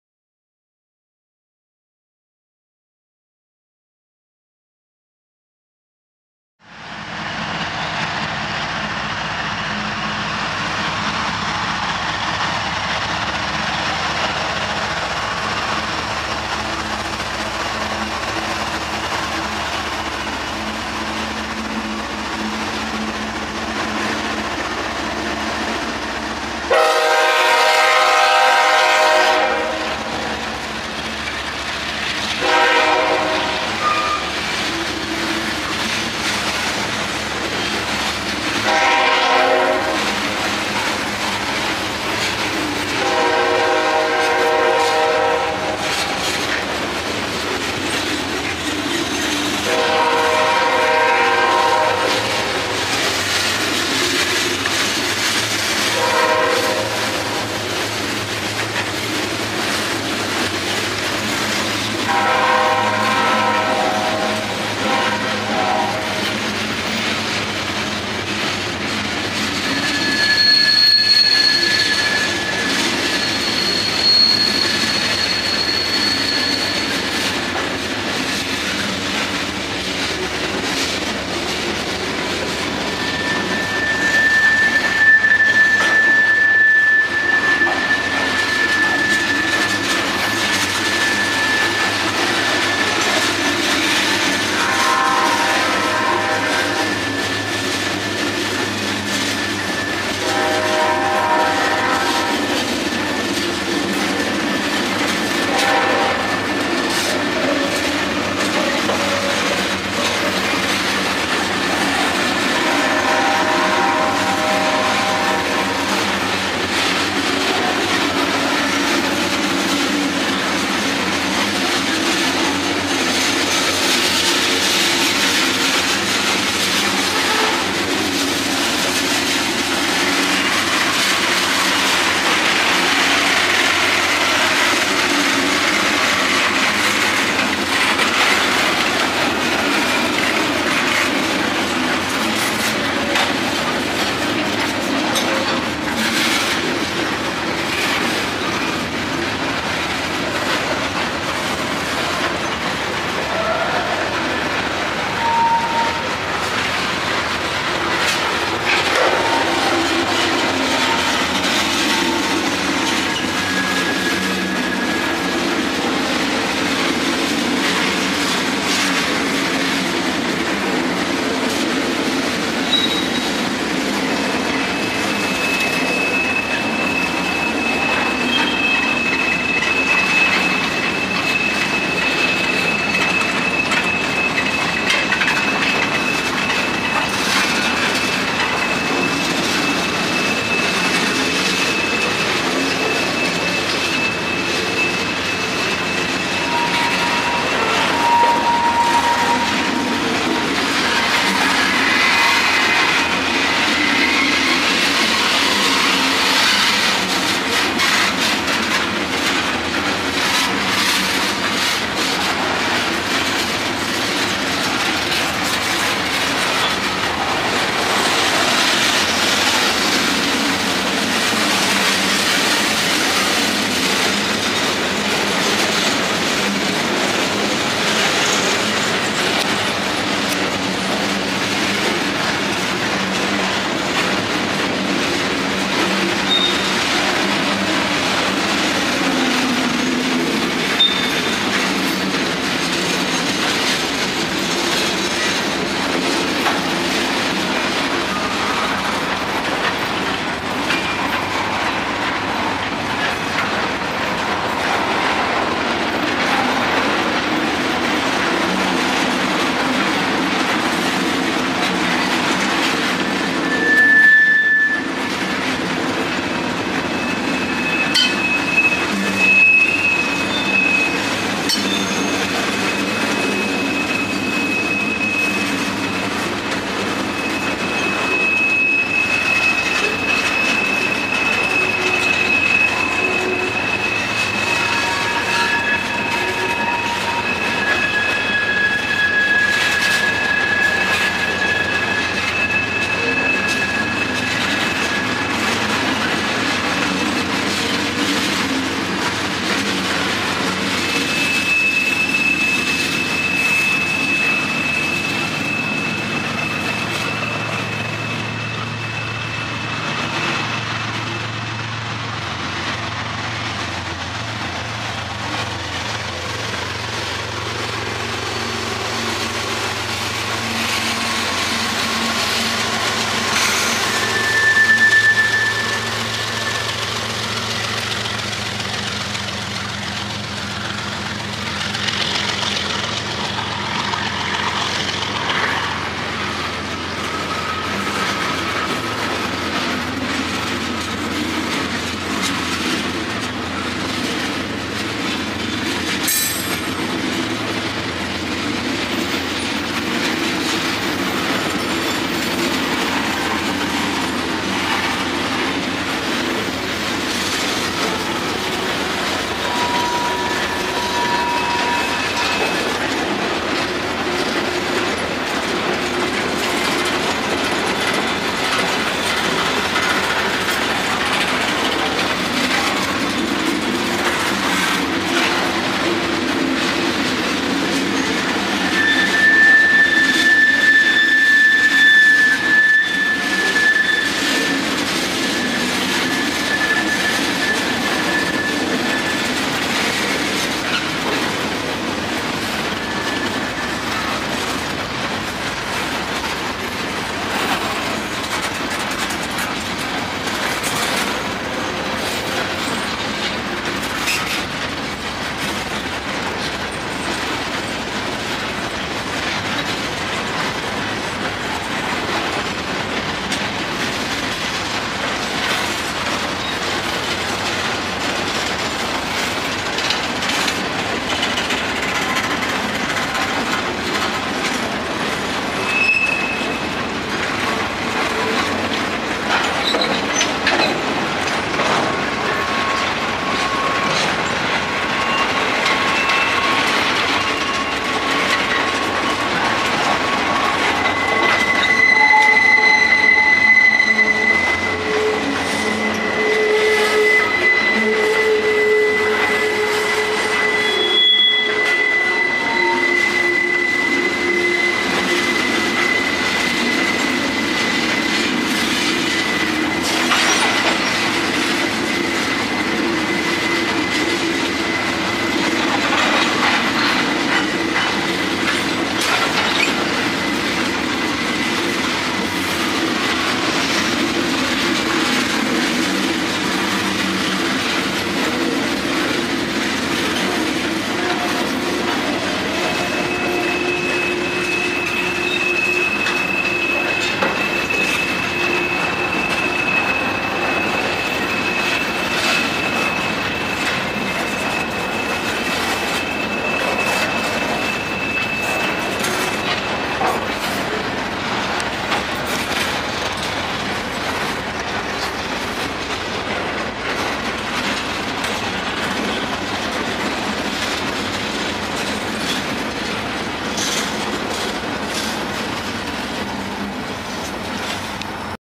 CSX Manifest Train MID DPU 1976 Conrail Augusta Ga
Got word that there was another CSX Heritage in town and not for long. Since I was only able to get Stills Of The Western Maryland on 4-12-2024, I decided to make my way to Downtown Augusta.

After about a 35-45 minute wait, they started going over the Manifest but didn't mention a DPU, just 107 cars.

I tried to find the best shaded spot, and as the Mixed Freight was heading out Northbound on the McCormick Sub Waycross Georgia halfway through, there was the 1973 Heritage Conrail

Our apologies if the video is a little shaky as we are getting used to a new camera. Thank you for understanding.

Waycross Georgia to Russelville Kentucky

CSX 3273 Leads
CSX 5251
CSX DPU 1976 Conrail

0:00 Intro
0:10 Locomotives W/Mixed Freight Departing For Russellville Ky
5:02 1976 CSX Conrail Heritage DPU